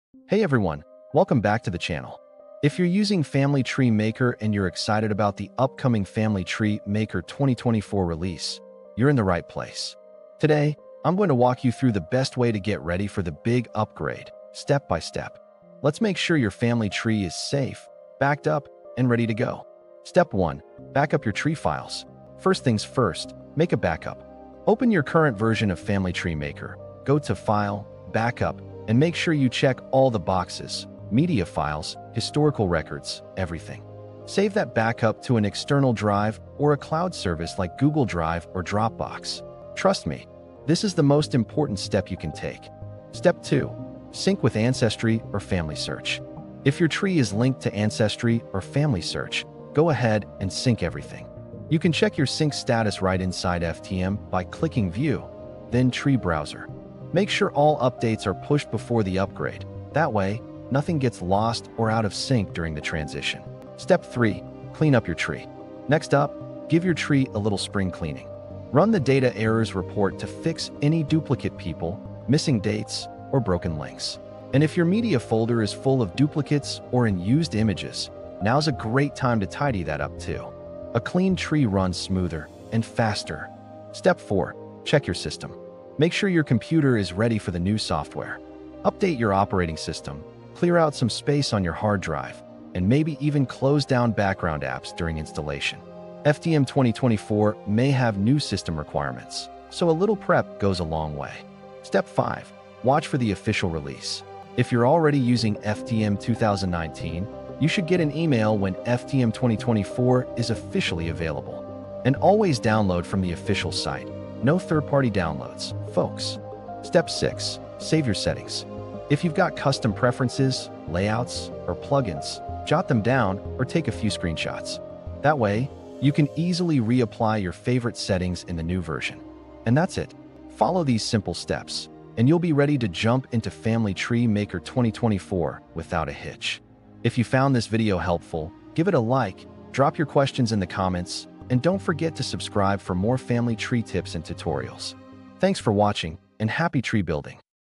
How to Prepare for Family Tree Maker 2024 Upgrade – Complete Step-by-Step Guide
The new Family Tree Maker 2024 is almost here! 🚀 Before you upgrade, it’s essential to make sure your family tree is safe, synced, and ready. In this video, I’ll show you exactly how to prepare so you can upgrade without stress or surprises.

Here’s what we’ll cover:
✅ How to back up your Family Tree Maker files
✅ Syncing with Ancestry & FamilySearch
✅ Cleaning up errors and duplicate media
✅ Checking FTM 2024 system requirements
✅ Saving your settings & preferences
✅ What to expect on release day

Whether you’re upgrading from FTM 2019 or switching from an older version, this guide will help you protect your research and ensure a smooth transition.

📌 Helpful Links:
🌐 Official Family Tree Maker Website
📩 Sign up for FTM 2024 release updates: FTM Email Sign-Up

💬 Have a question about upgrading? Drop it in the comments — I’m here to help!